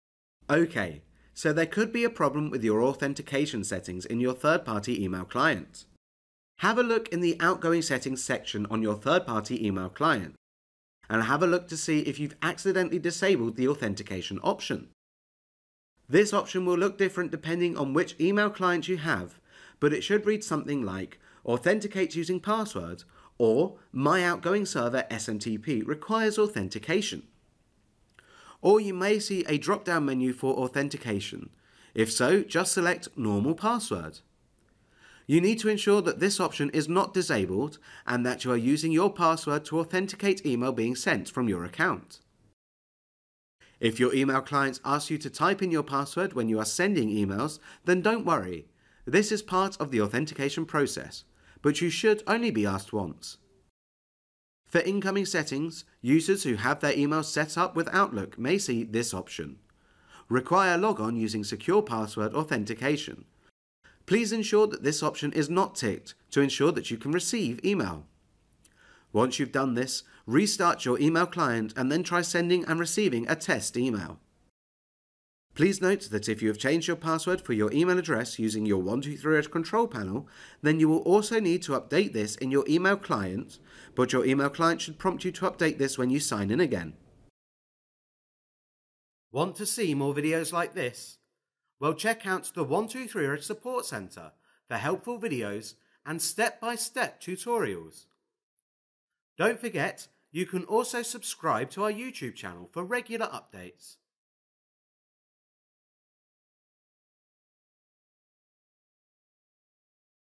Is your authentication option disabled? | 123-reg
If you are having trouble sending and receiving email, watch this video to see if maybe this is caused by incorrect authentication settings in your 3rd party email client.

Check the outgoing settings section in your 3rd party email client to see if maybe you have accidentally disabled the authentication option. Depending on which email client you are using, this option could read something like “authenticate using password” or “my outgoing server (SMTP) requires authentication”.

You will need to ensure that this option is enabled and that you are using a password to authenticate email being sent from your account.

Once you’ve made these changes, simply restart your email client and try sending and receiving a test email to ensure it’s working.

For complete steps on how to check and change these settings, watch this video or read our support article